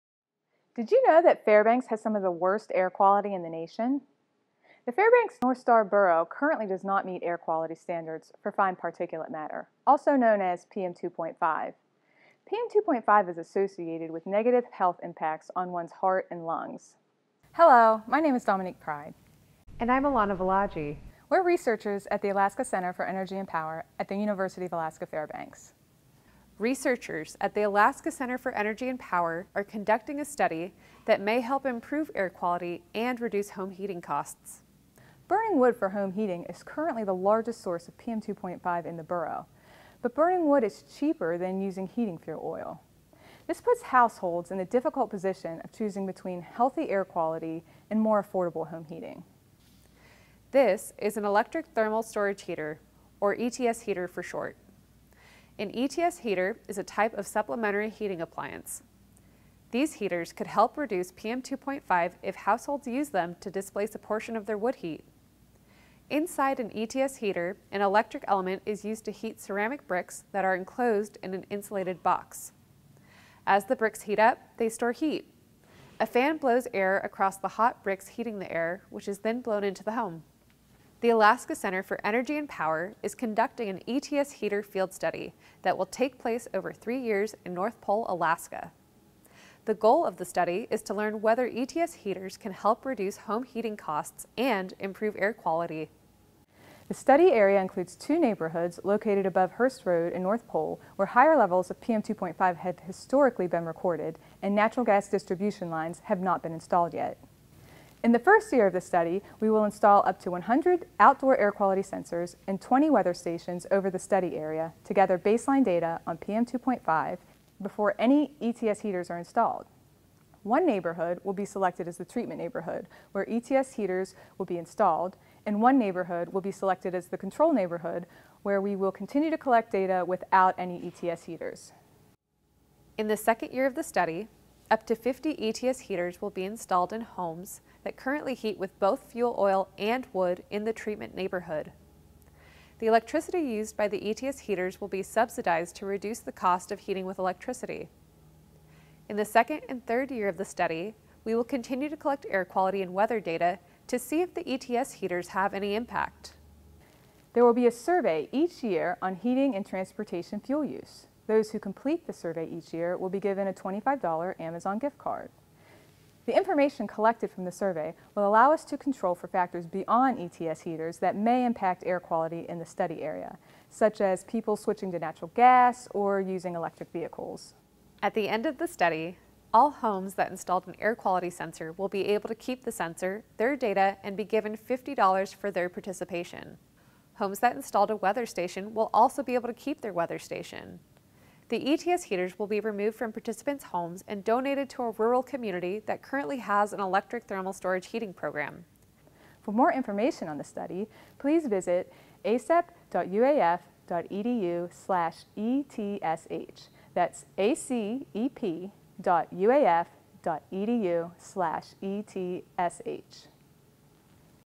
This residential heater could help the Fairbanks North Star Borough improve air quality
The Alaska Center for Energy and Power is conducting an electric thermal storage heater field study that will take place over three years in North Pole, Alaska. The goal of the study is to learn whether electric thermal storage heaters,  a type of supplementary heating appliance, can help reduce home heating costs and improve air quality, especially PM2.5.

Burning wood for home heating is currently the largest source of PM2.5 in the borough. But burning wood is often cheaper than using heating fuel oil. This puts many households in the difficult position of choosing between healthy air quality and more affordable home heating.

An electric thermal storage heater is a type of supplementary heating appliance, and these could help reduce PM2.5 if households use them to displace a portion of their wood heat.

Find out more about the program at acep.uaf.edu/etsh.